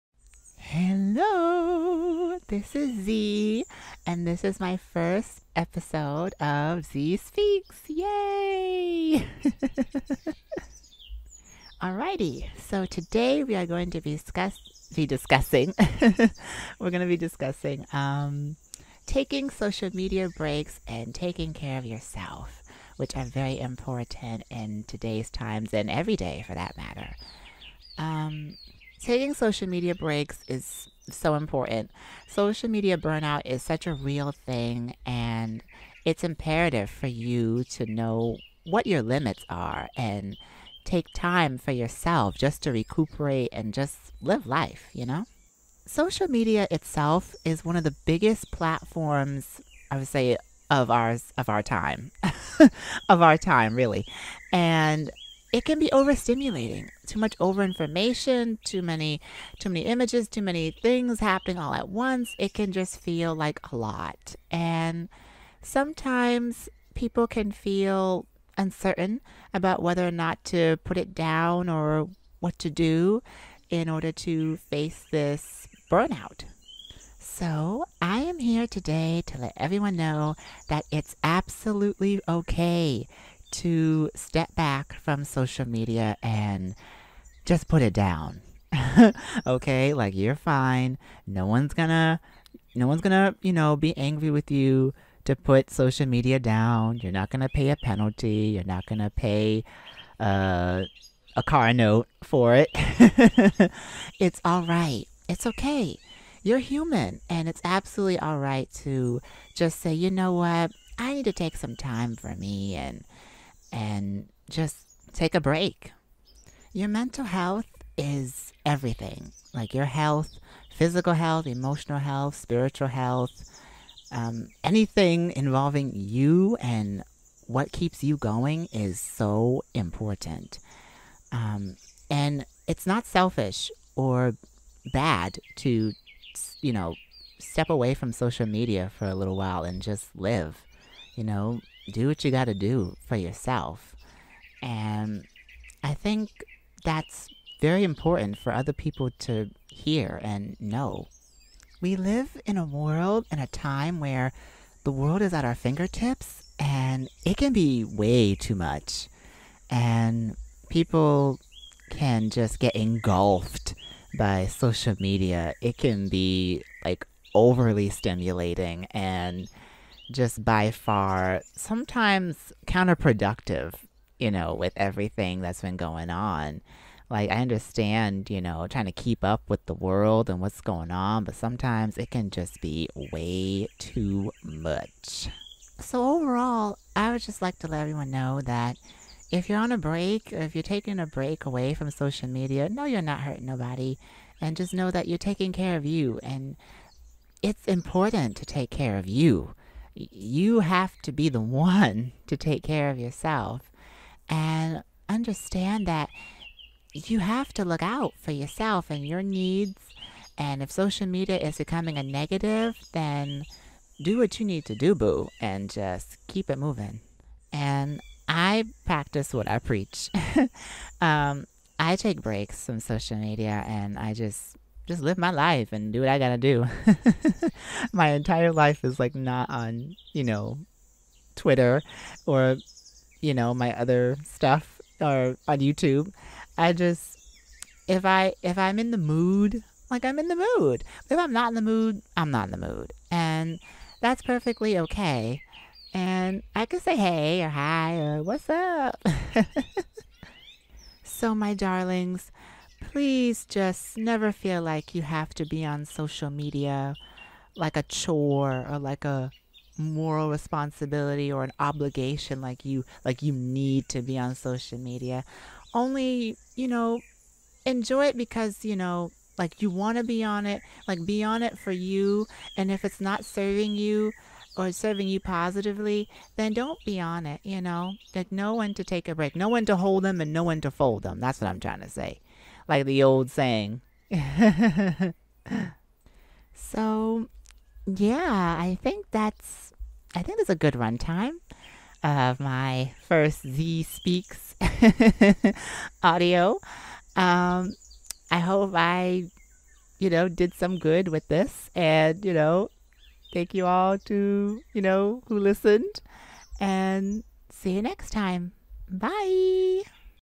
Z Speaks EP#1-Social Media Breaks [M4A] [ASMR] (Taking Care Of Your Mental Health) (Affirmations)
Hello hello! It's Z, and this is my first ever Z Speaks. It's an ASMR-styled Podcast where I talk about taking care of yourself and self-care affirmations. 100% improv and no script, just from my mind and my thought process. Enjoy!